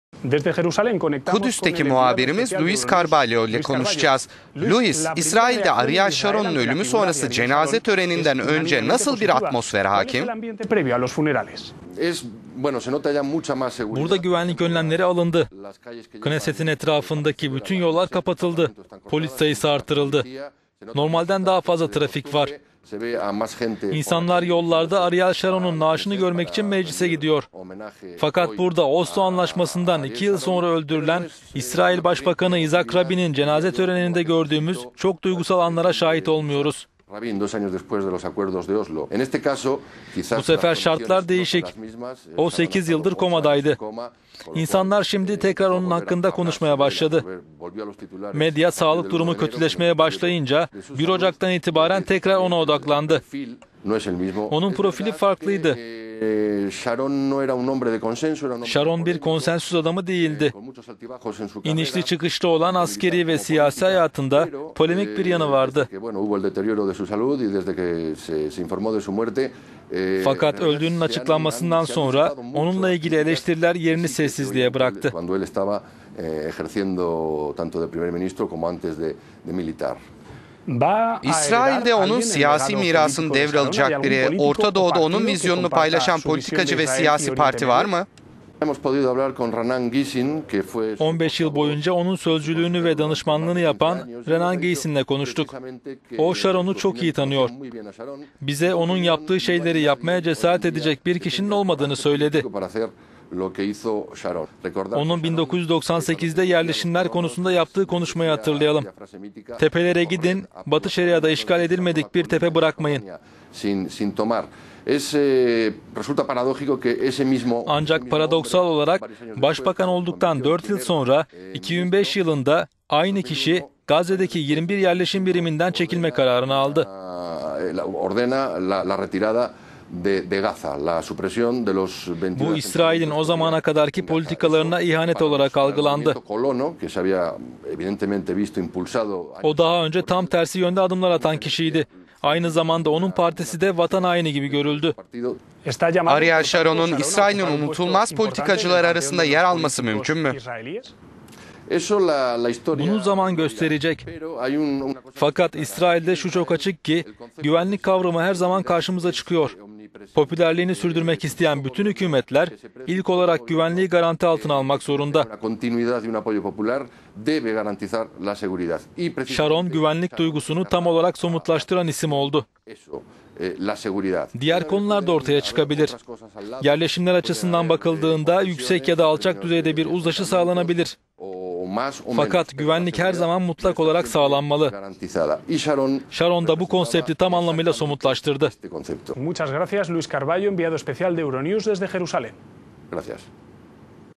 İsrail Ariel Şaron'un cenaze törenine hazırlanıyor
Kudüs'teki muhabirimiz Luis Carballo ile Ariel Şaron sonrasını konuştuk.

euronews: ...

euronews:
"Şaron'un ölümü sonrası cenaze töreninden önce ülkede nasıl bir atmosfer hakim?"

Luis Carballo, euronews:
"Burada güvenlik önlemleri alındı. Knesset'in etrafındaki bütün yollar kapatıldı. Polis sayısı arttırıldı. İnsanlar yollarda Ariel Şaron'un naaşını görmek için meclise gidiyor. Fakat burada Oslo Anlaşması'ndan iki yıl sonra öldürülen İsrail Başbakanı İzak Rabin'in cenaze töreninde gördüğümüz çok duygusal anlara şahit olmuyoruz. Bu sefer şartlar değişik. O sekiz yıldır komadaydı. İnsanlar şimdi tekrar onun hakkında konuşmaya başladı. Medya, sağlık durumu kötüleşmeye başlayınca, 1 Ocak'tan itibaren tekrar ona odaklandı. Onun profili farklıydı. Şaron, bir konsensüs adamı değildi; inişli çıkışlı olan askeri ve siyasi hayatında polemik bir yanı vardı. Fakat öldüğünün açıklanmasından sonra onunla ilgili eleştiriler yerini sessizliğe bıraktı."

euronews:
"İsrail'de onun siyasi mirasını devralacak biri, Ortadoğu'da onun vizyonunu paylaşan politikacı ve siyasi parti var mı?"

Luis Carballo, euronews:
"15 yıl boyunca onun sözcülüğünü ve danışmanlığını yapan Renan Gysin ile konuştuk. O Şaron'u çok iyi tanıyor. Bize onun yaptığı şeyleri yapmaya cesaret edecek bir kişinin olmadığını söyledi. Onun 1998'de yerleşimler konusunda yaptığı konuşmayı hatırlayalım: "Tepelere gidin. Batı Şeria'da işgal edilmedik bir tepe bırakmayın." Ancak paradoksal olarak, başbakan olduktan dört yıl sonra 2005 yılında, aynı adam Gazze'deki 21 yerleşim biriminden çekilme kararını aldı. Bu İsrail'in o zamana kadarki politikalarına ihanet olarak algılandı. O daha önce tam tersi yönde adımları atan kişiydi. Aynı zamanda onun partisi de vatan haini gibi görüldü."

euronews:
"Ariel Şaron'un İsrail'in unutulmaz politikacıları arasında yer bulması mümkün mü?"